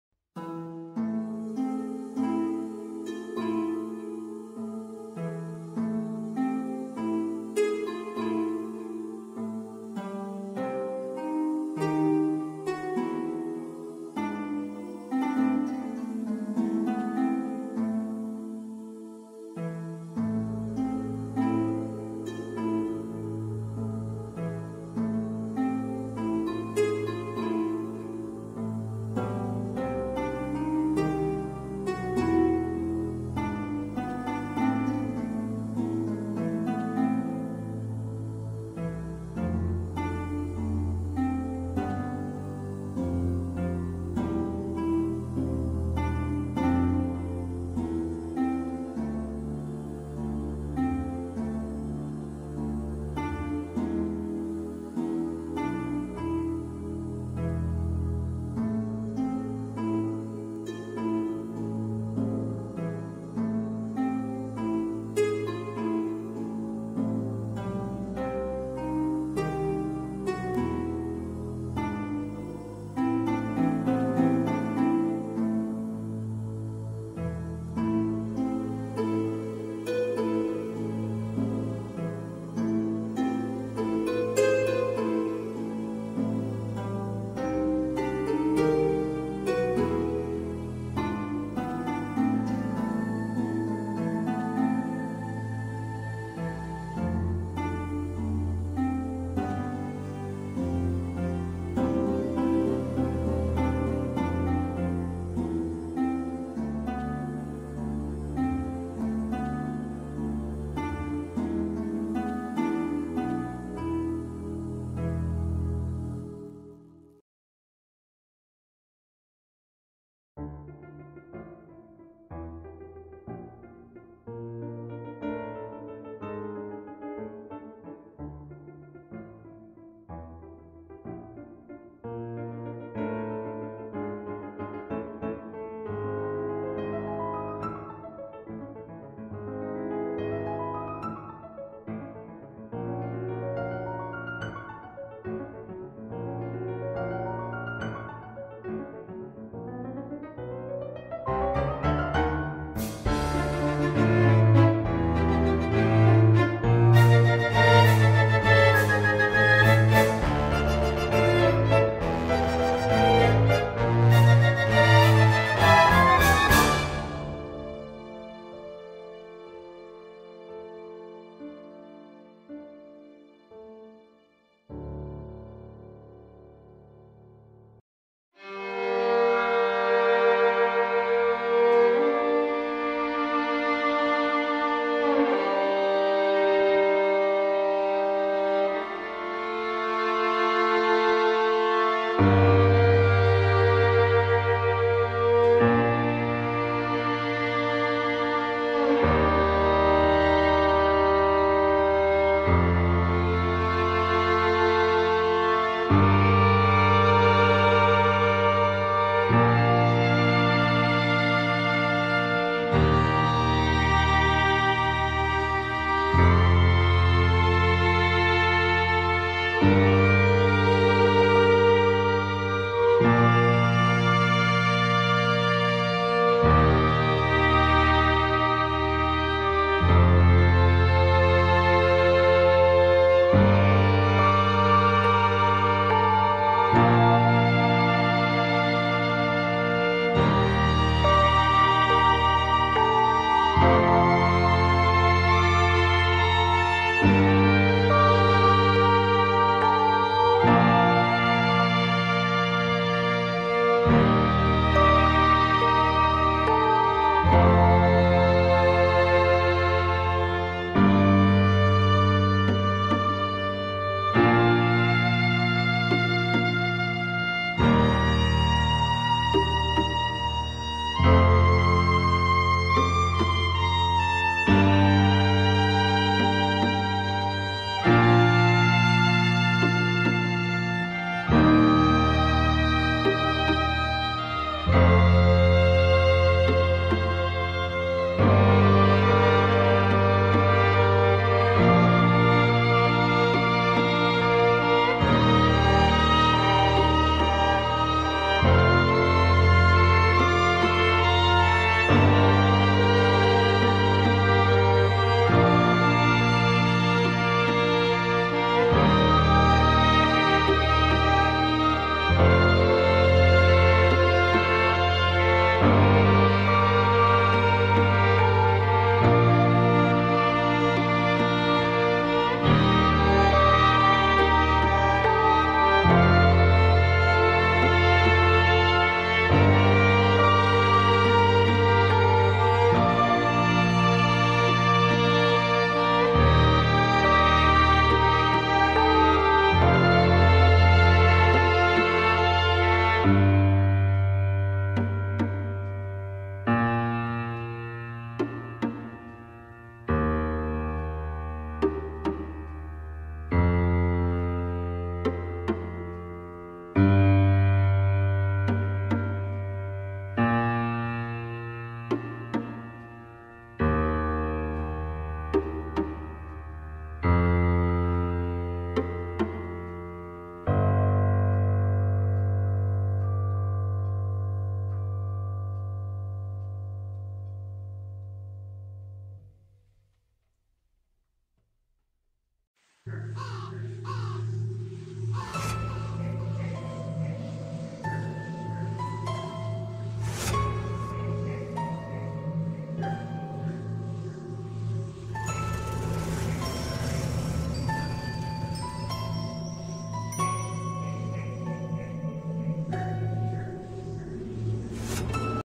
31 days of Halloween watercolor painting day 30
Watercolor speedpainting - Friendly ghost. Thrirtyeth picture of the 31 days of Halloween challenge.